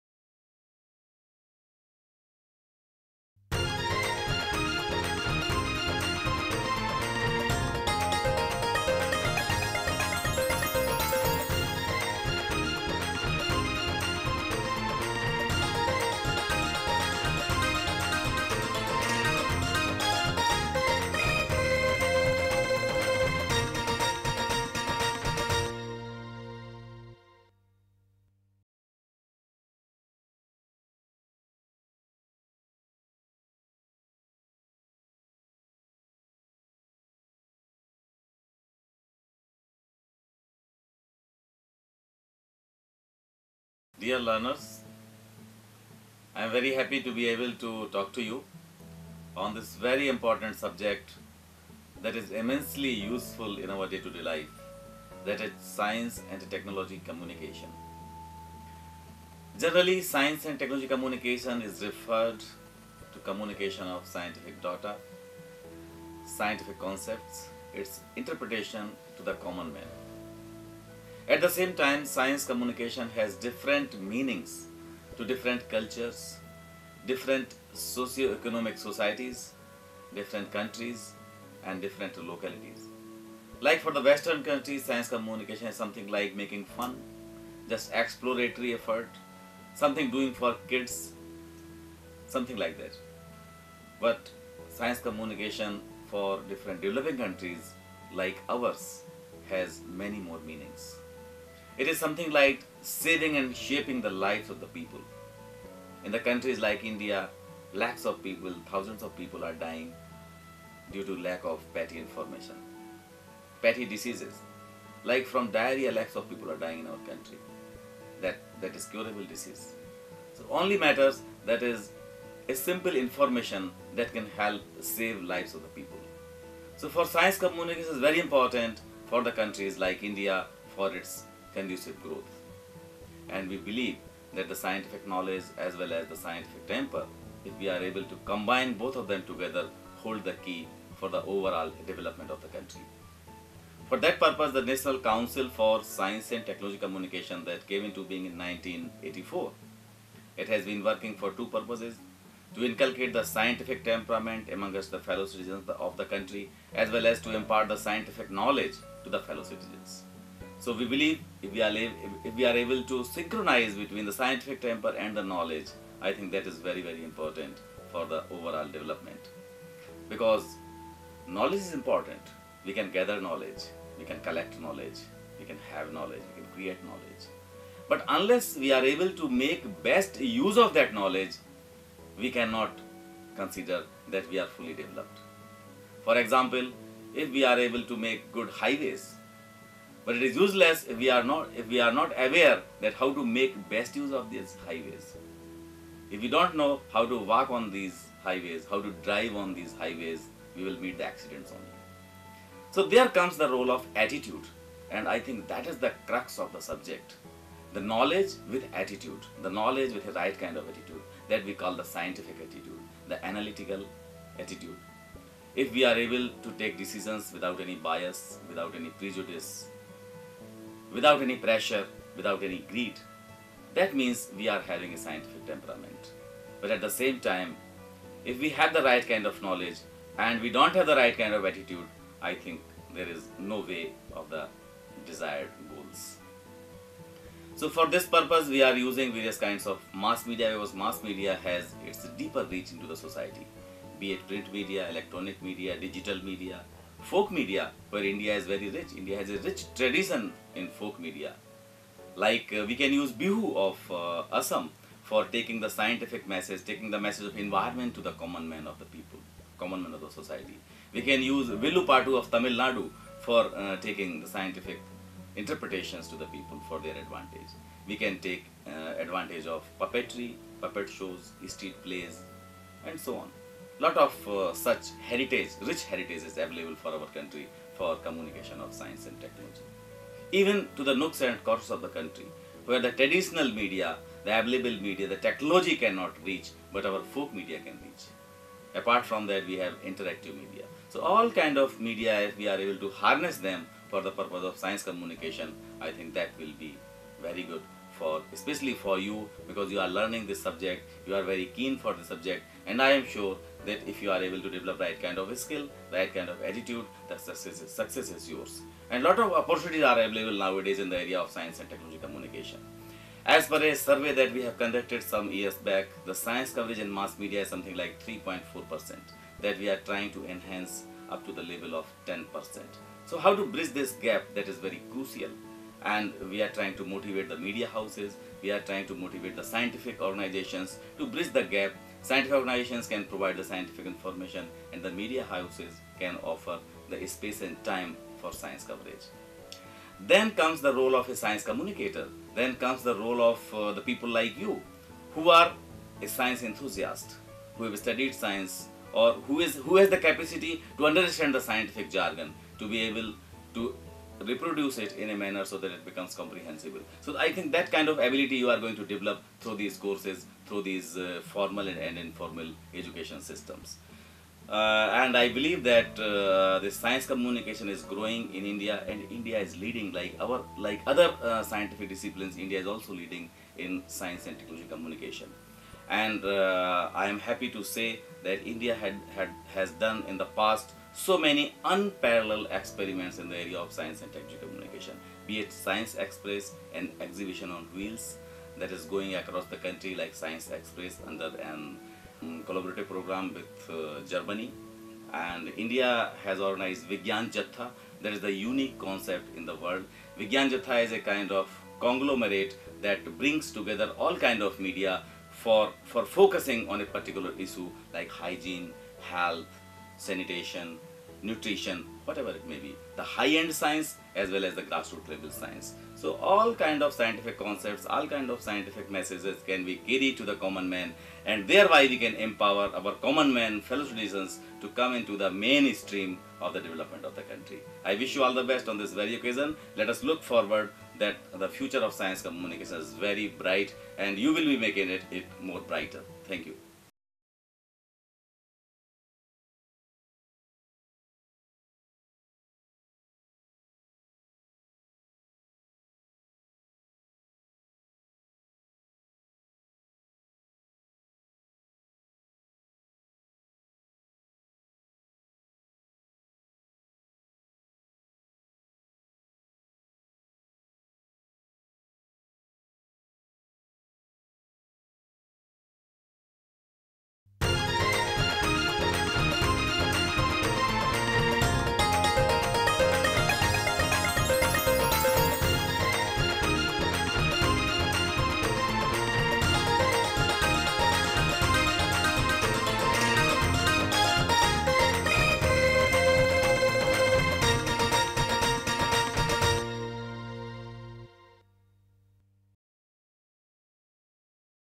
Science Communication
In this programme, Dr. Manoj Patairiya, Director of NCSTC has presented a lecture on science communication, its importance, the benefits and how it can mould the views of people regarding our society and environment. He laid emphasis on the fact that more and more journalists should report science news and try to bring it to the mainstream media. The programme is produced by audio visual project of Krishna Kanta Handiqui State Open University, Guwahati, Assam, India.